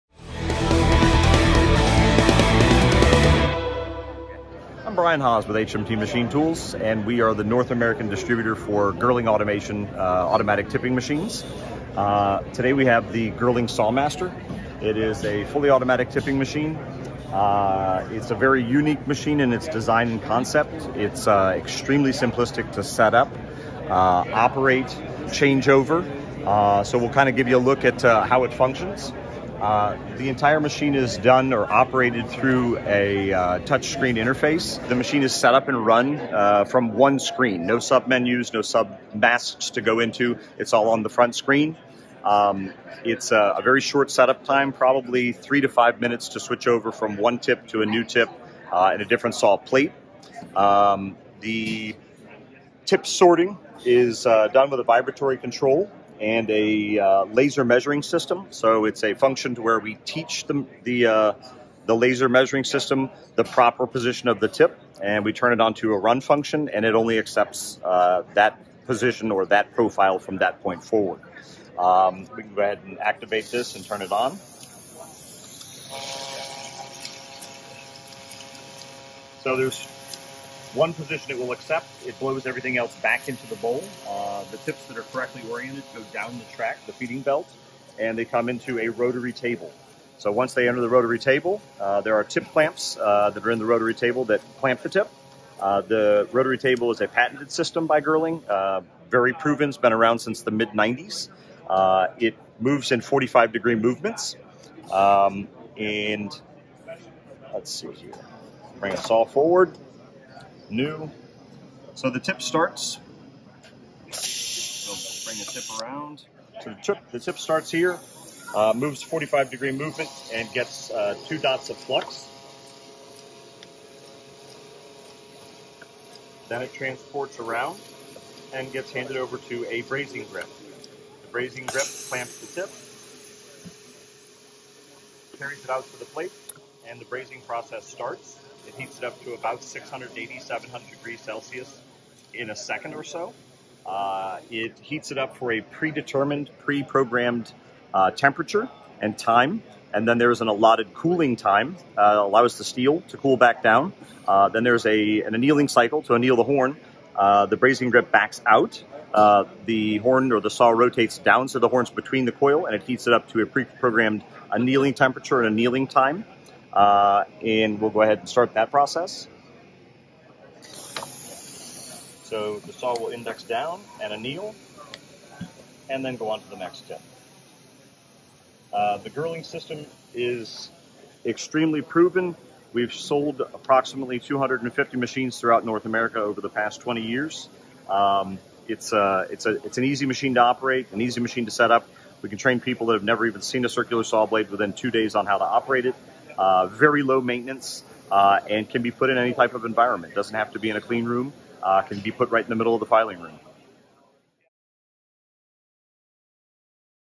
Automatic tip brazing with Gerling and HMT Machine Tools
HMT Machine Tools' Brian Haas outlines the features of the Gerling Automation SawMaster automatic tip brazing machine at the 2022 BC Saw Filers Association convention trade show.